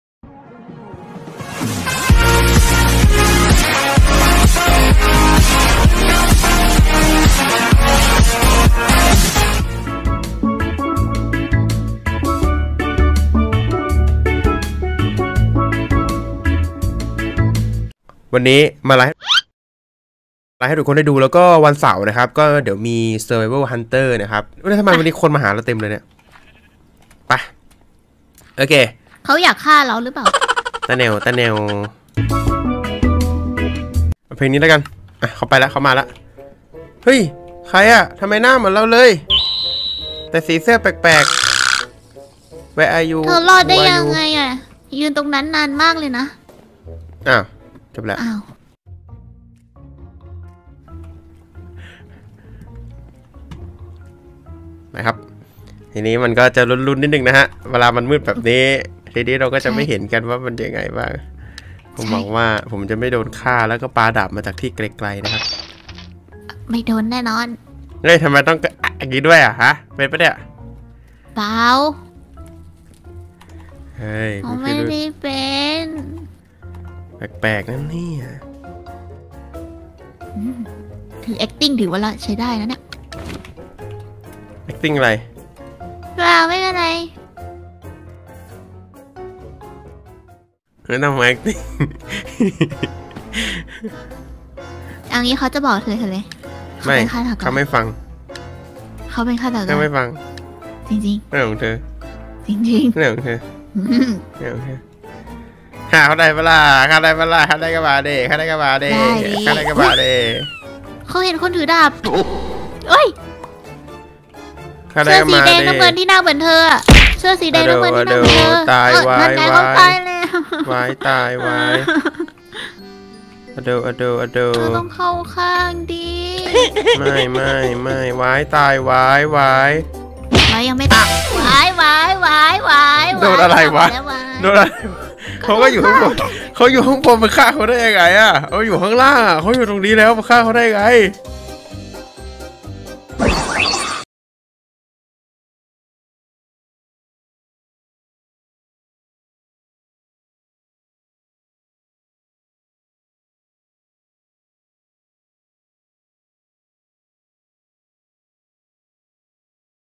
เล่นมินิเกมฆาตกรเจอ พี่ไวท์กับพี่บี Deklaaon ครั้งแรก
[▼ อยากใช้ Cape AlbyPro Magma ซื้อได้ที่นี่นะครับ ▼]

Thumbnail by:
EDITING SOFTWARE: Kinemaster
RECORDING SOFTWARE: